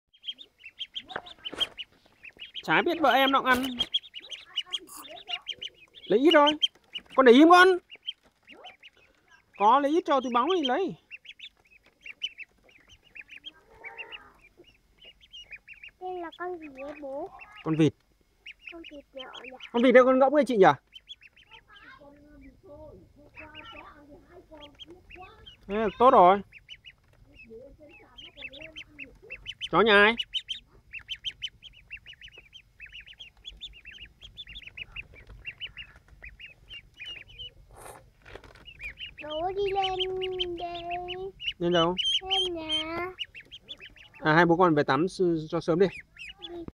Cute baby ducklings
The newly hatched ducklings, they are hung up by the owner to fight the attacking dogs. Last night four children were killed by a dog